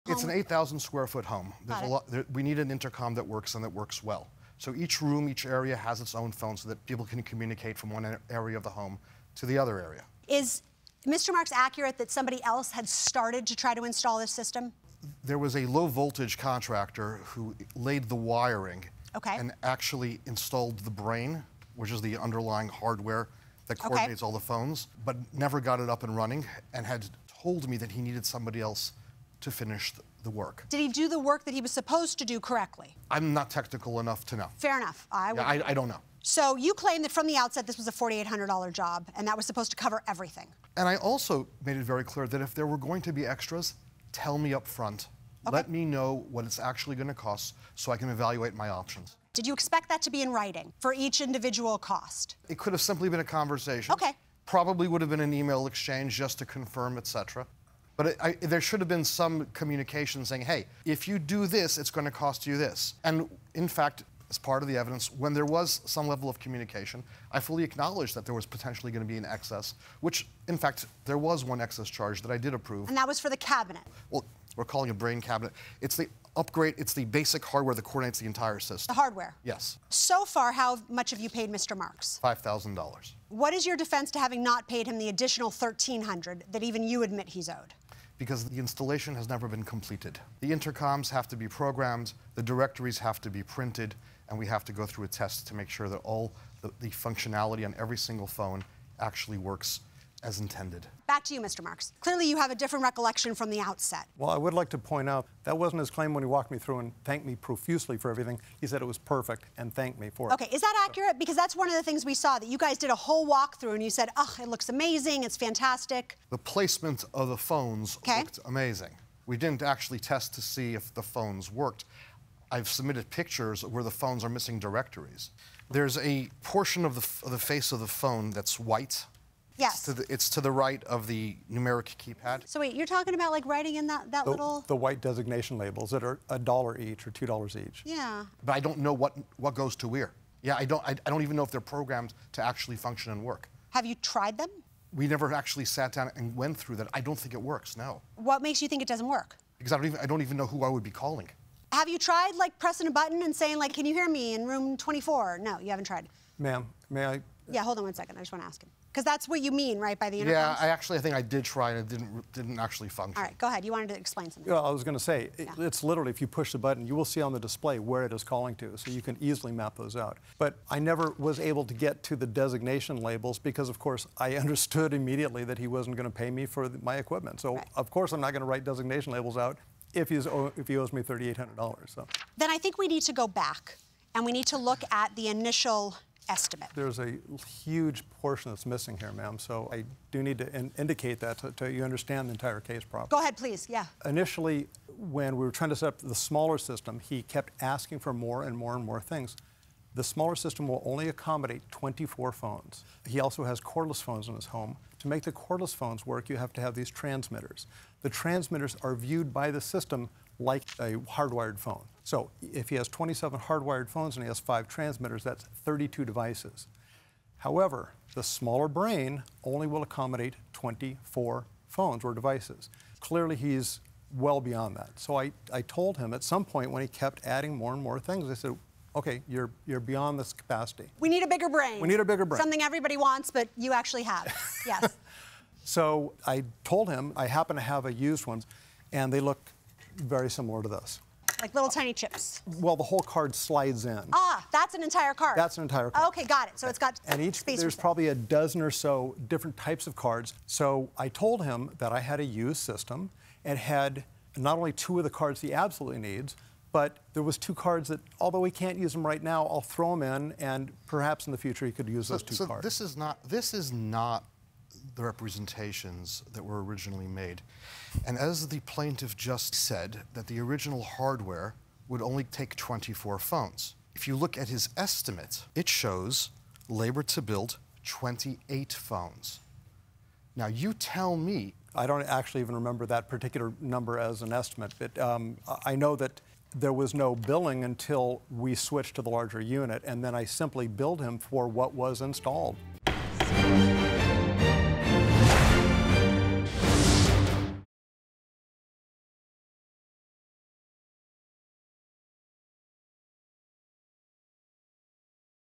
Client Is Hung Up on Phone System Invoice! | Part 1
Spencer claims his client Yosef added phones to his order after his initial quote, and now Yosef refuses to pay additional costs!